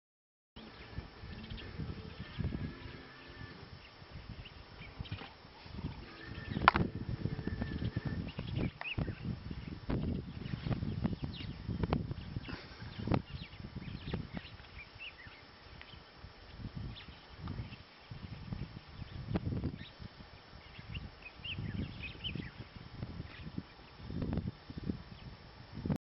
Zebra - hit on the nose
Hitting a young one on the mouth